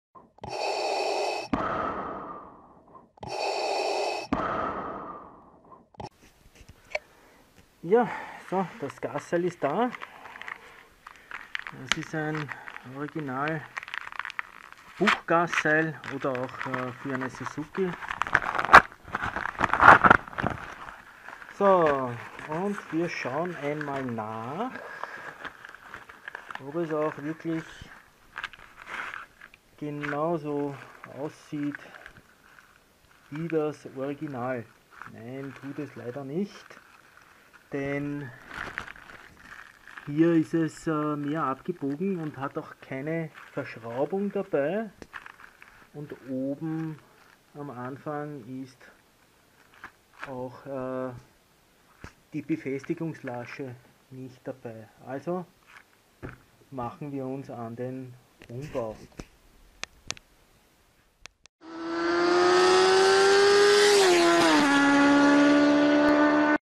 Neues originales Gasseil -- New original acceleration cable
Das ist das neue und originale Gasseil für die Puch Lido (gleiche Ersatzteilnummer). Allerdings passt es nicht als Gasseil, sondern als Seilzug für den Choke.
This is the new and original acceleration cable for the Puch Lido (same spare part number). However, it does not fit as a acceleration cable, but as a cable for the choke.
E1